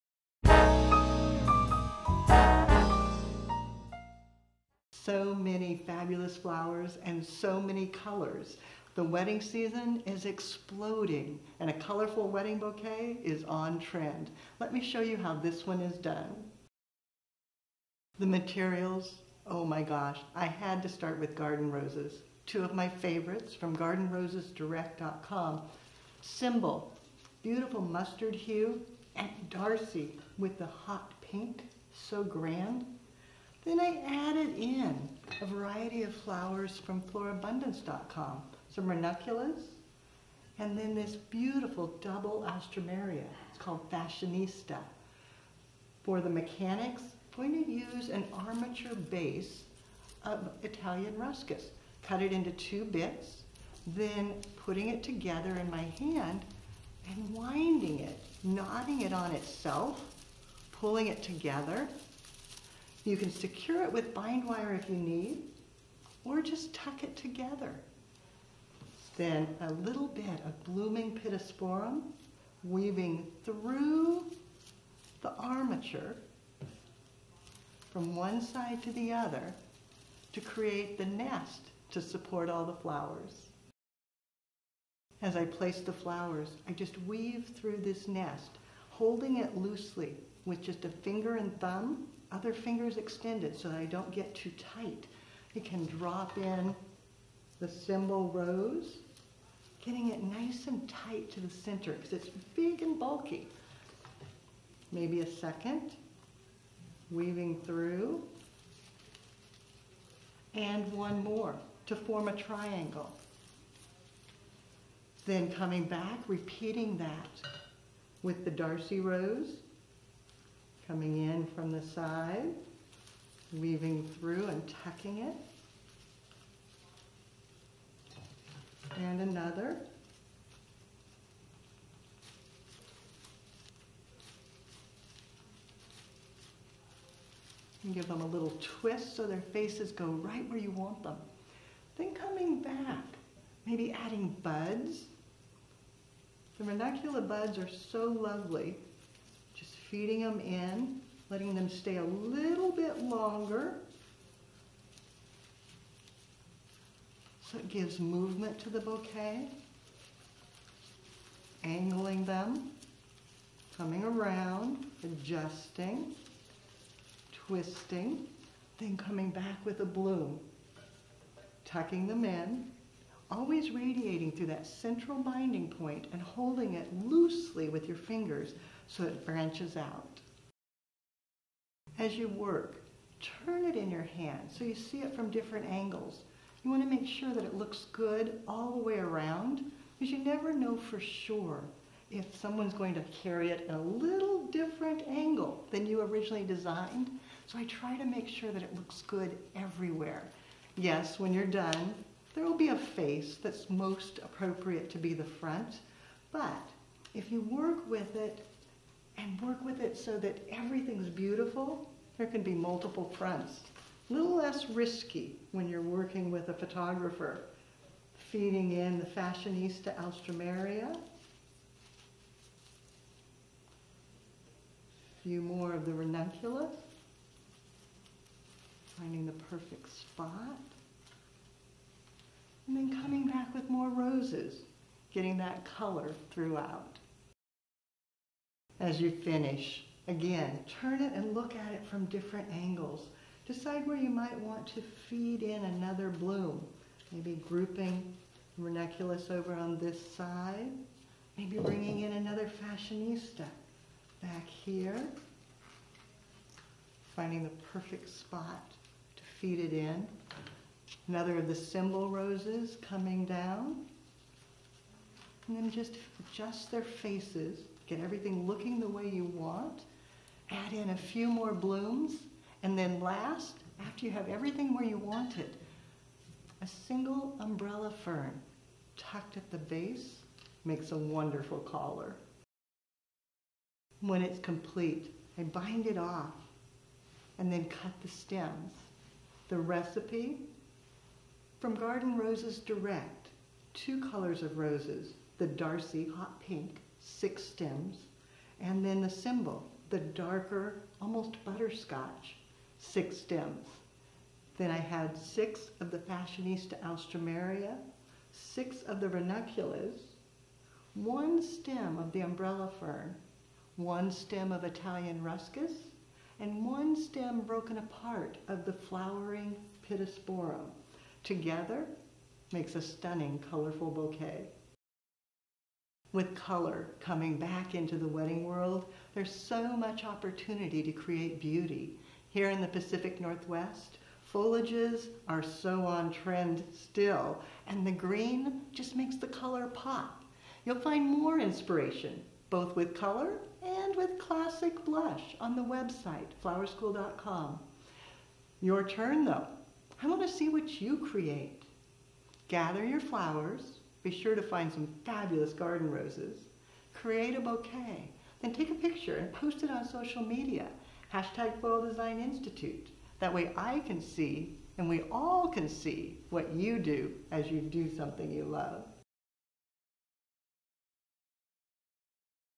Colorful Wedding Bouquet
So many fabulous flowers and so many colors! The wedding season is exploding, and a colorful wedding bouquet is on trend. Watch how this bouquet using Symbol and Darcey (Auschariot) garden roses is created by Leanne Kesler of Floral Design Institute.